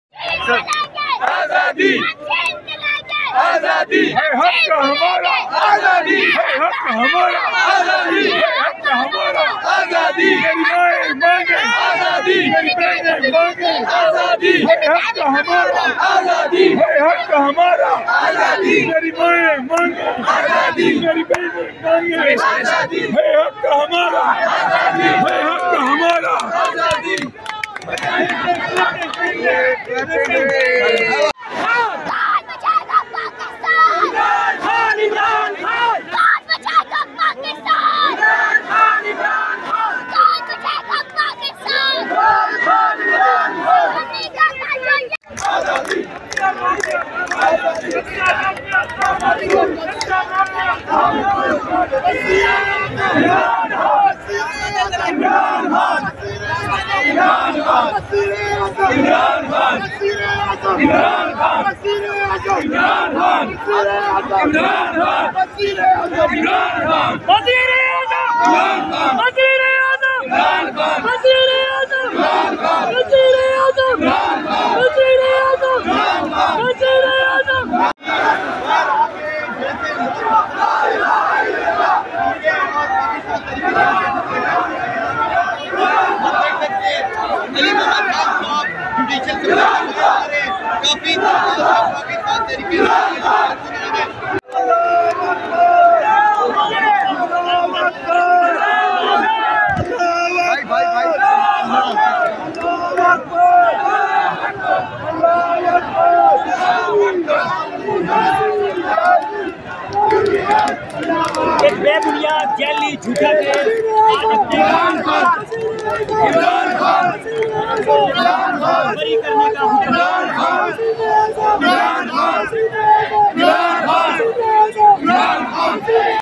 Footage of Latif Khosa & PTI Workers Celebrating Massive Victory in Their Final Case | Dawn News
Latest Visuals Of PTI Leaders' Celebration Following Victory In Iddat Case | Dawn News

عدت کیس میں بشریٰ بی بی اور عمران خان بری
عدالت کے باہر پی ٹی آئی کا جشن، خصوصی مناظر